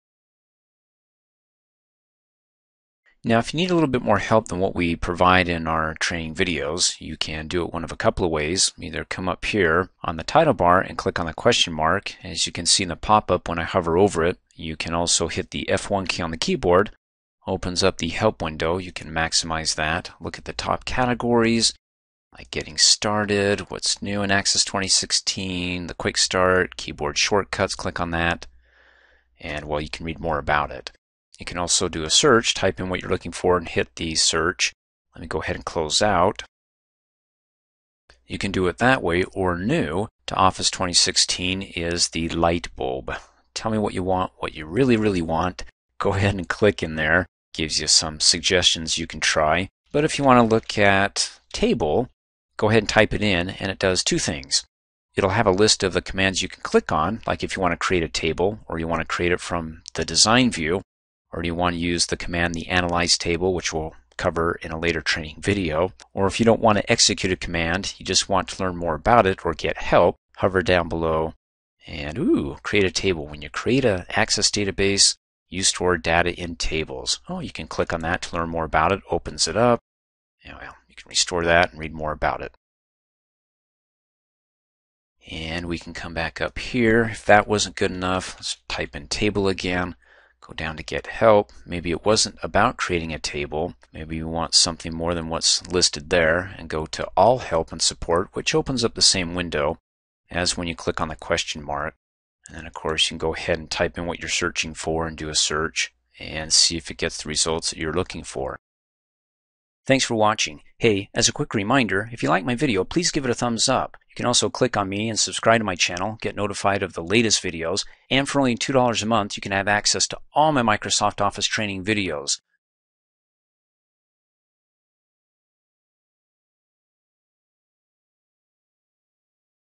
Access 2016: New Tell Me What You Want Or Help Feature
Access 2016 training video on the new Tell Me What You Want that is part of the Help feature, and is used to find answers to questions about features in Access, as well as execute commands.